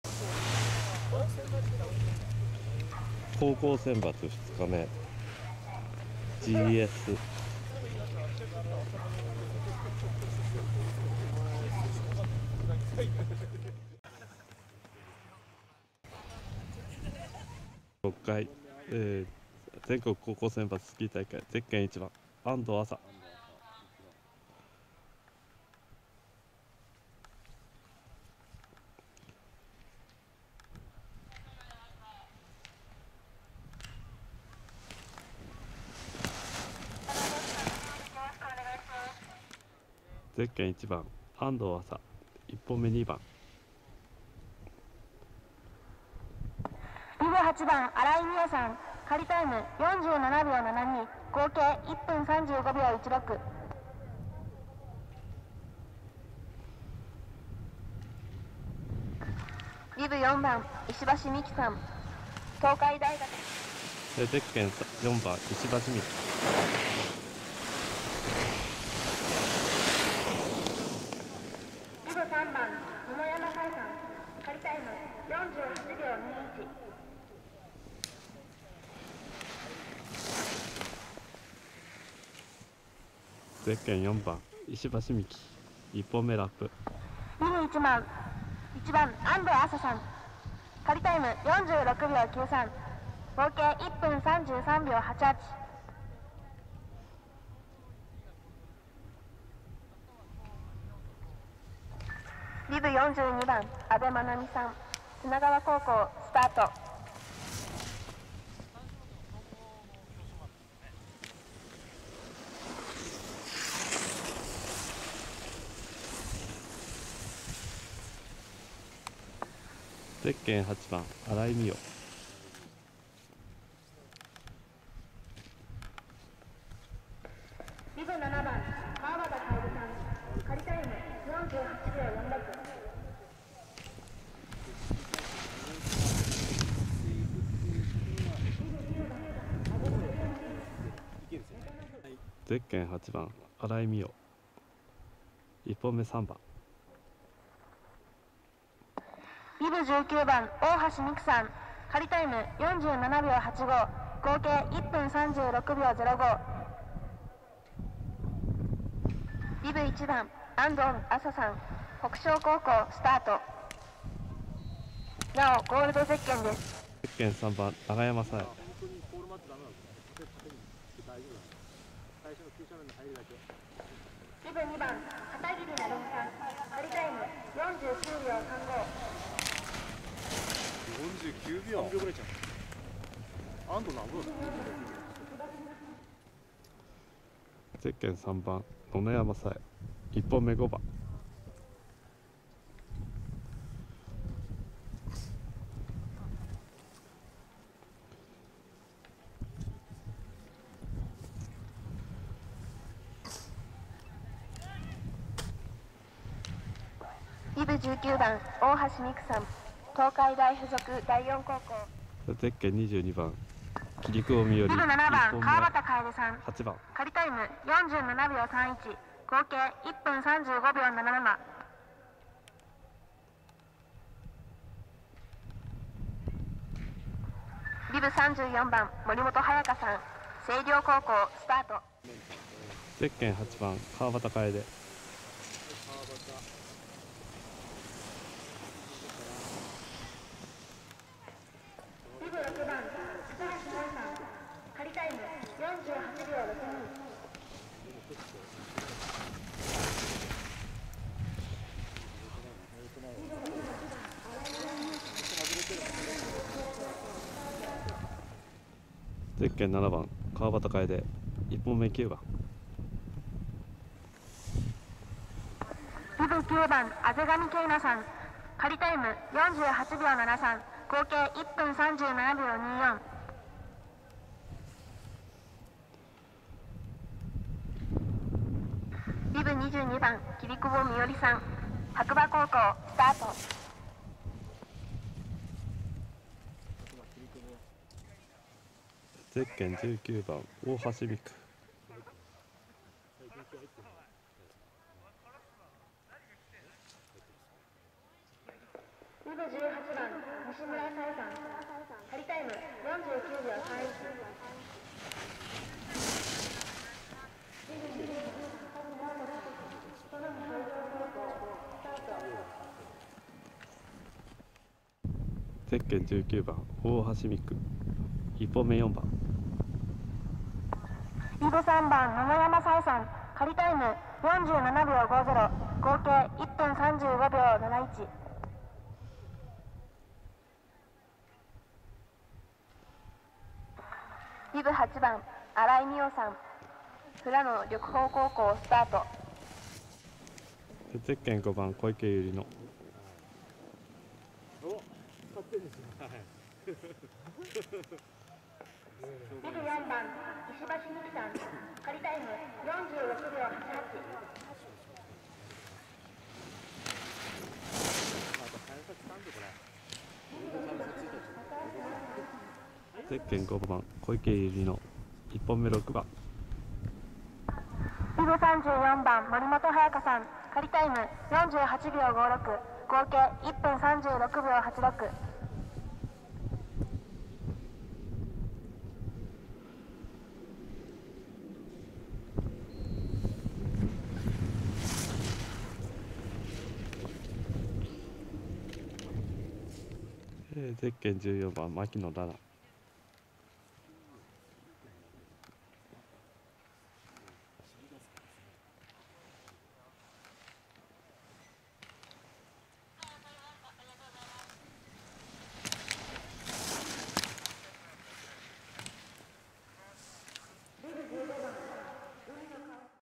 第２６回全国高等学校選抜スキー大会 女子GS入賞者
平成26年3月15日富良野スキー場で開催された「第26回全国高等学校選抜スキー大会」女子GSの入賞者
1位　安藤麻　2位　石橋未樹　3位　荒井美桜　4位　野々山颯絵
5位　切久保美織（２本目のみ）　6位　川端楓　8位　大橋海紅　9位　小池百合乃
10位　牧野レナ（１本目のみ）
※7位の林崎華帆は撮りそびれた